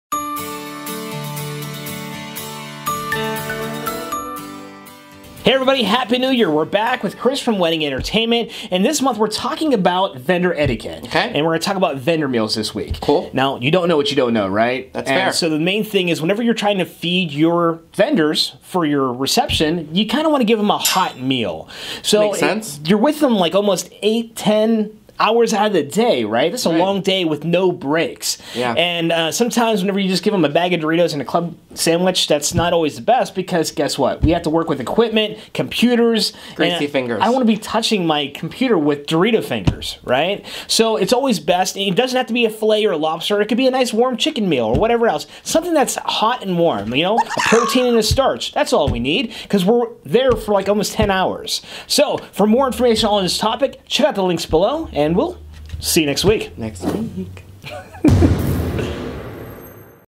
Do We Have to Feed Our Wedding Vendors? | Wedding Tips & Planning
Do we have to feed our wedding vendors? What do you feed wedding vendors?

We publish new Wedding Tips every Wednesday.